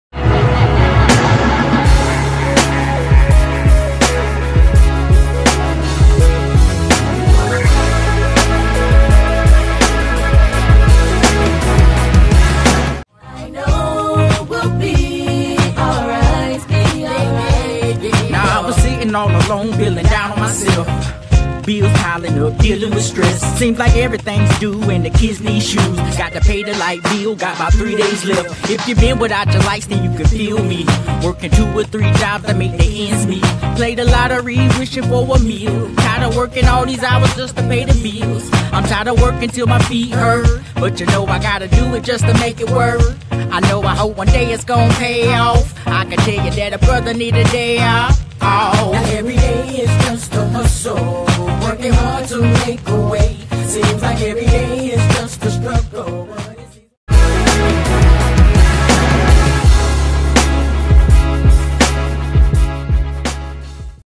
O.Shariff Performs at Coast 2 Coast LIVE | Raleigh 5/13/19 - 1st Place
Monday, May 13, 2019 9:00 PM

The Pour House Music Hall